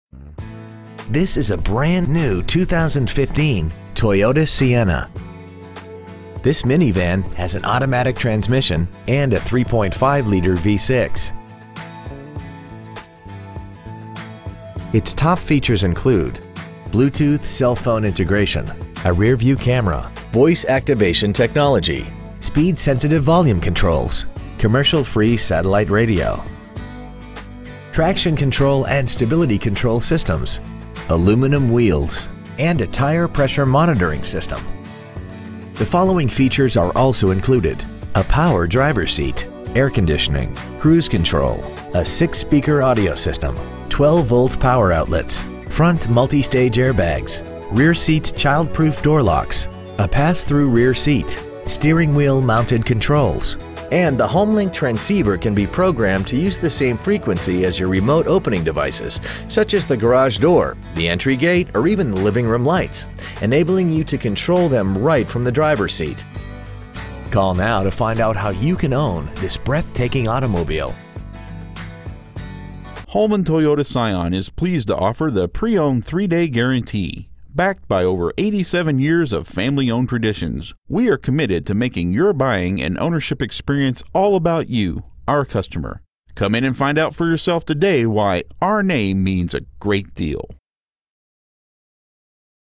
2015 Toyota Sienna Mount Laurel NJ 08054
We've been honored to serve the Mount Laurel NJ area , we promise that your experience at our dealership will exceed your expectations ! Year : 2015 Make : Toyota Model : Sienna Engine : Regular Unleaded V-6 3.5 L/211 Trans . : Automatic Exterior : Attitude Black Miles : 6 Interior : Bisque Stock : FS536097 Holman Toyota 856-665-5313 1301 Route 73 N Mount Laurel , NJ 08054